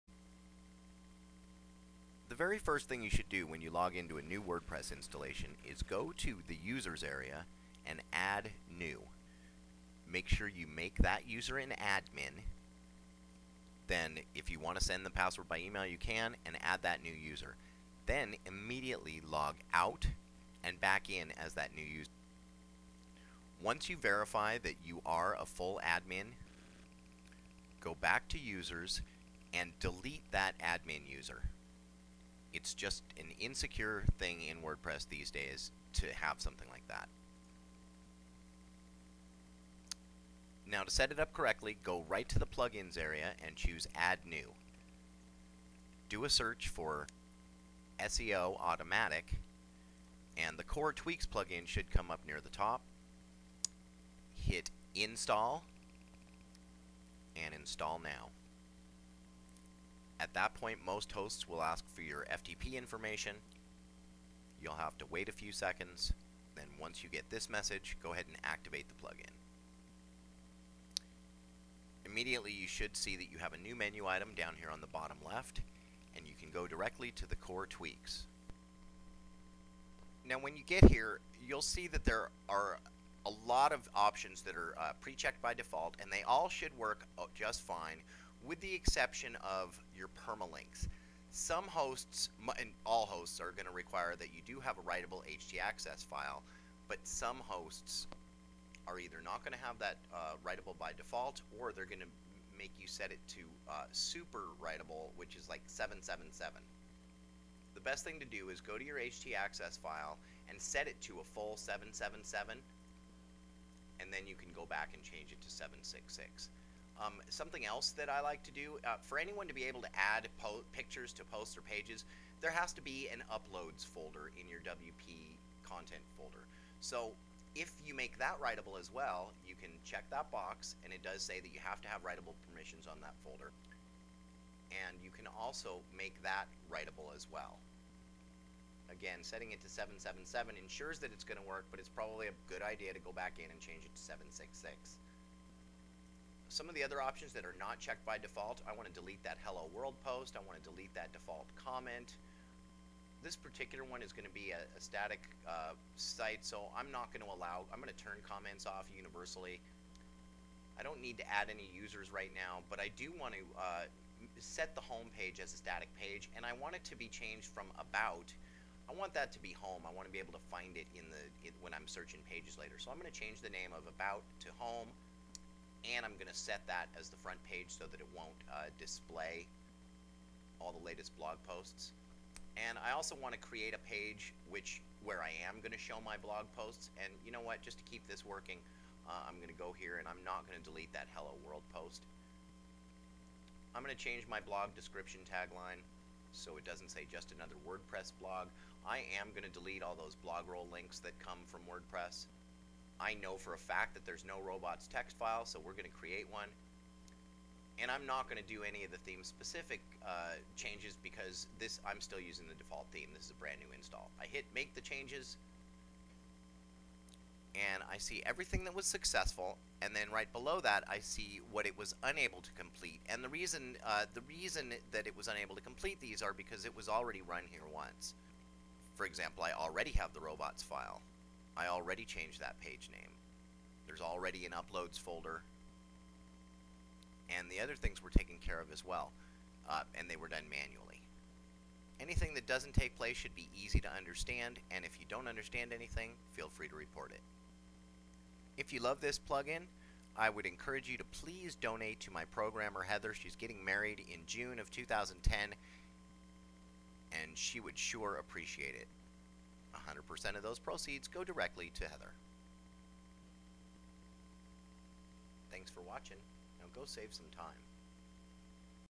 Save 20 minutes on every WordPress Setup
AFTER your WP installation, this plugin will save you AT LEAST 20 minutes, no matter how well you know WordPress, and no matter what you want to change.

Newbies can use the defaults, while experts will find all of the time consuming steps for "proper" setup placed in one simple admin panel with a one -click process.  Even the error messages have been made user friendly!

If you set up WordPress sites on a regular basis,  or you're just looking to do it once, you've got to see this...